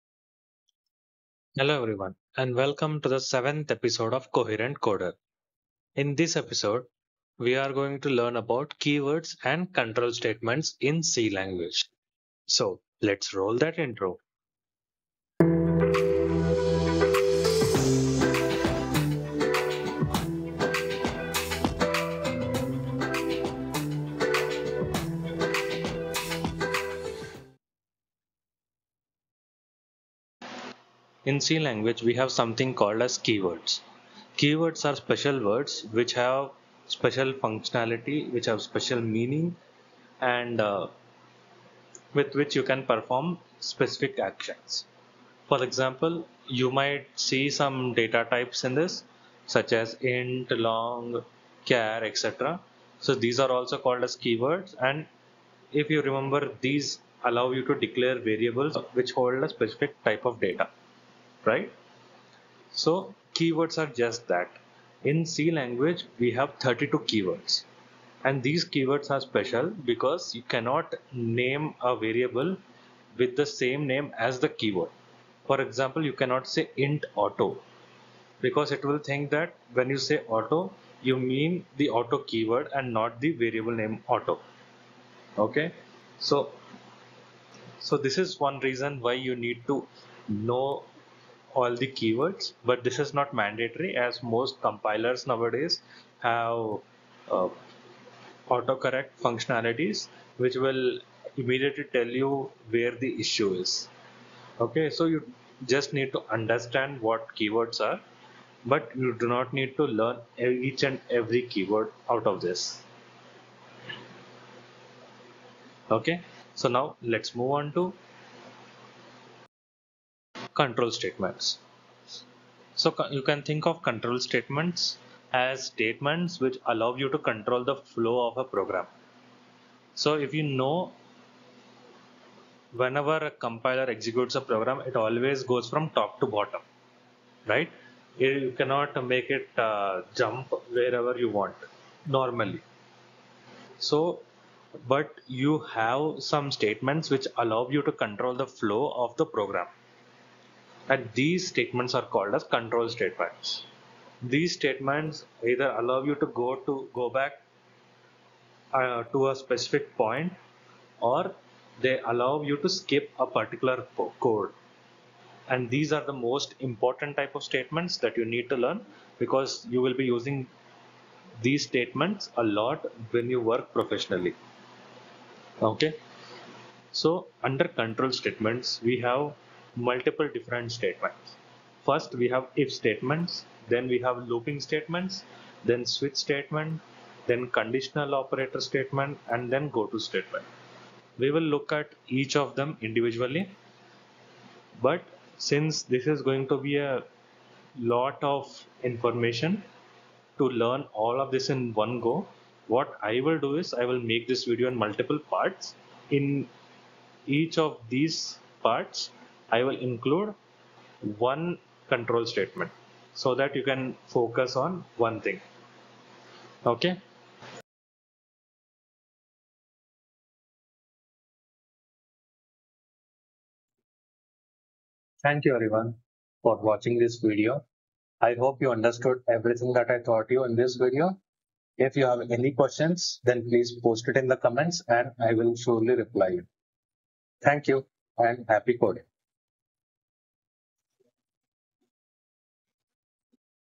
What are keywords and Control Statements in C Programming?
In this video i talk about what keywords and control statements are. I will discuss control statements in detail in the future videos.